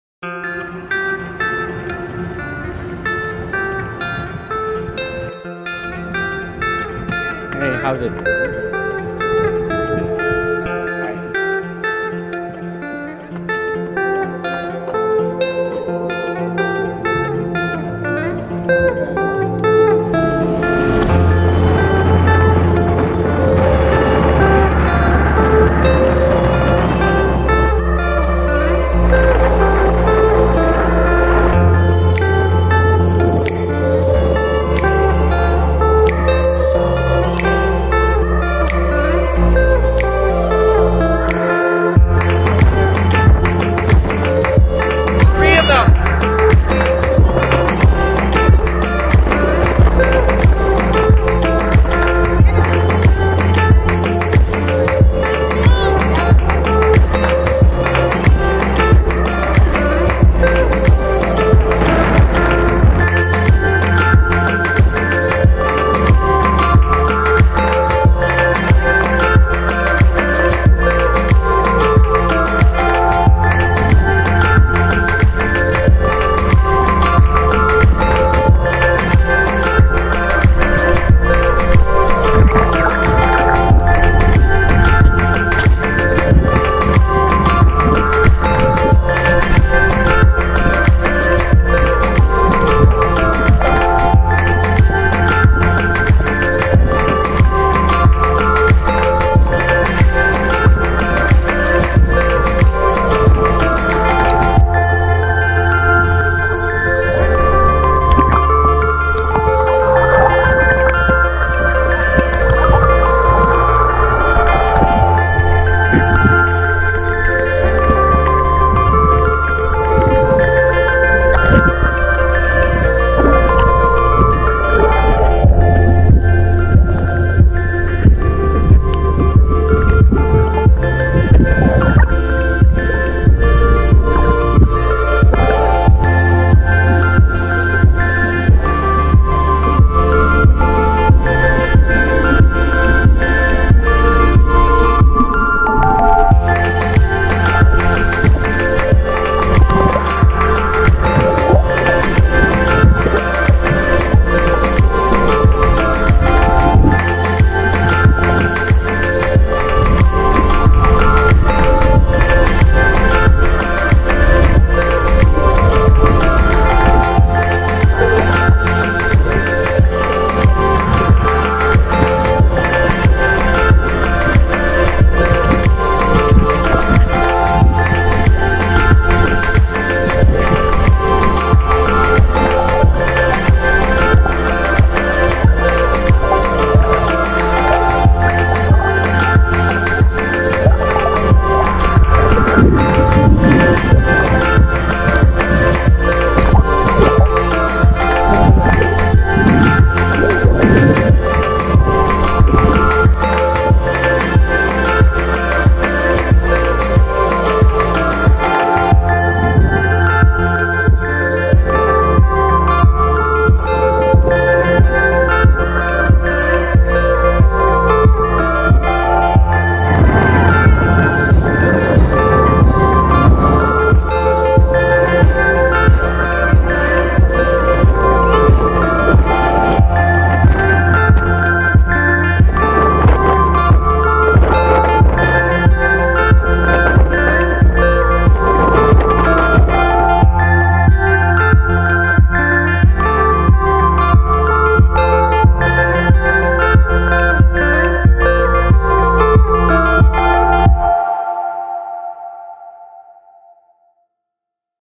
Swimming with the Sea Turtles off the Coast of Hawaii 2017
We decided to take a tour off the shores of Oahu aboard a $2 million dollar catamaran.  A 30 minute jaunt out from Honolulu into the Pacific Ocean and to Turtle Canyon. The highlight of the day was our swim with the many sea turtles!